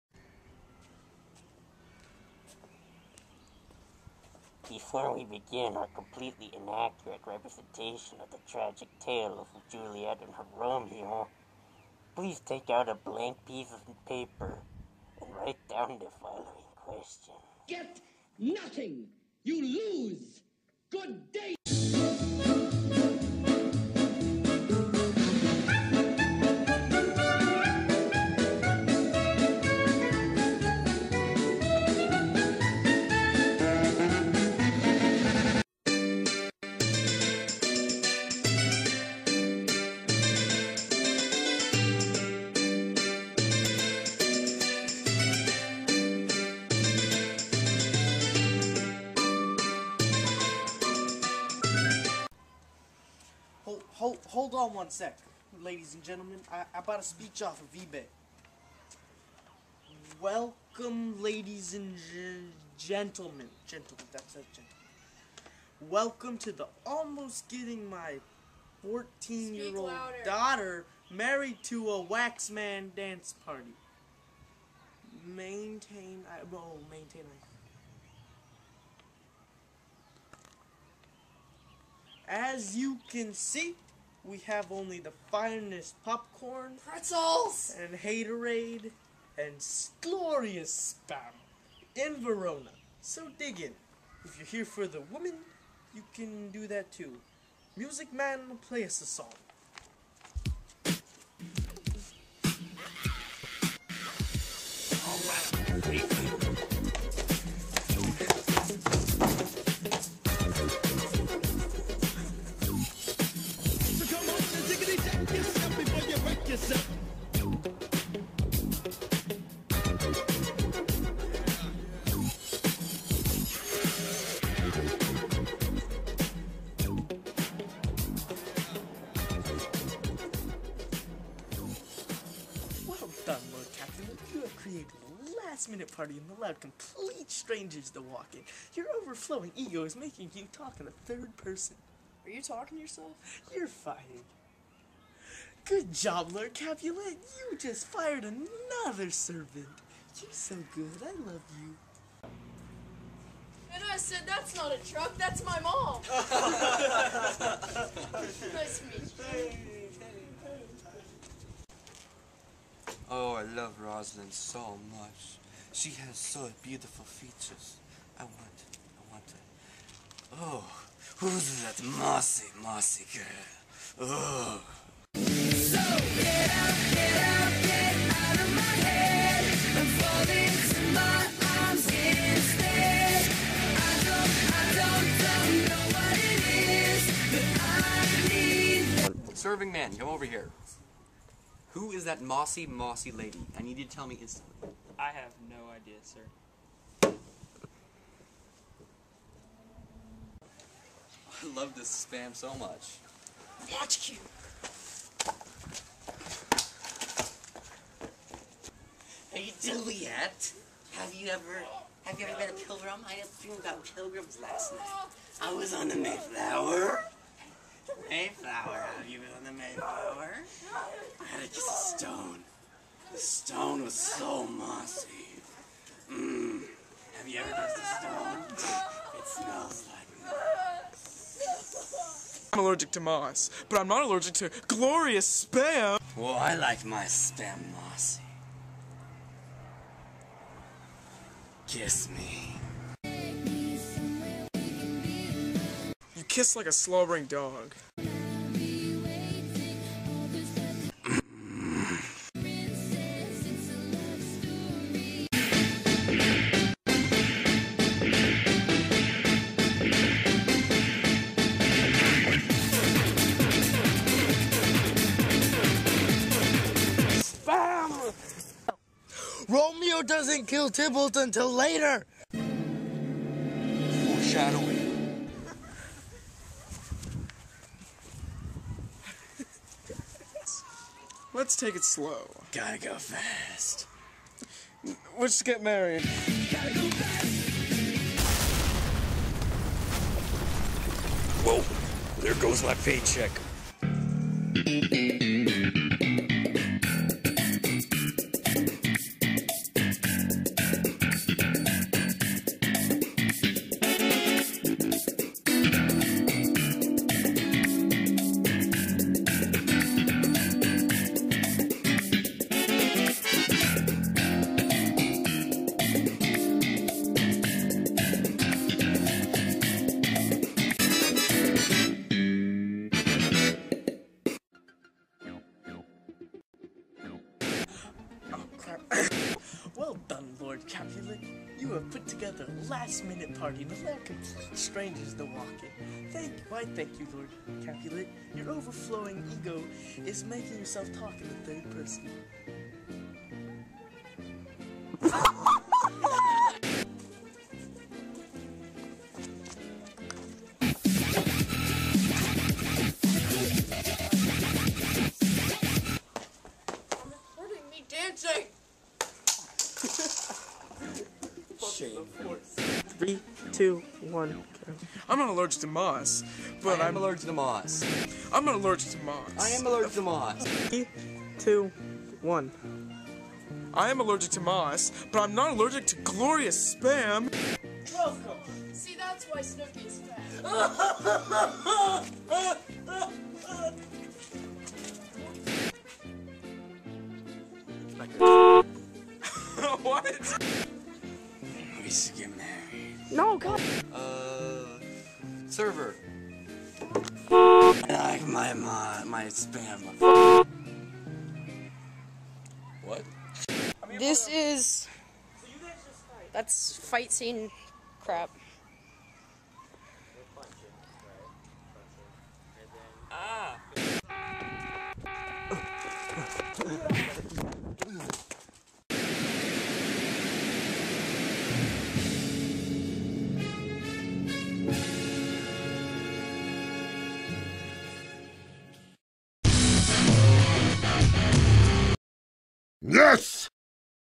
Romeo and Juliet Parody: Act 1 - Scene 5
It's love at first sight as Romeo meets Juliet. What could possibly go wrong?
Everything.
Made as a Final Project for my Honors English class.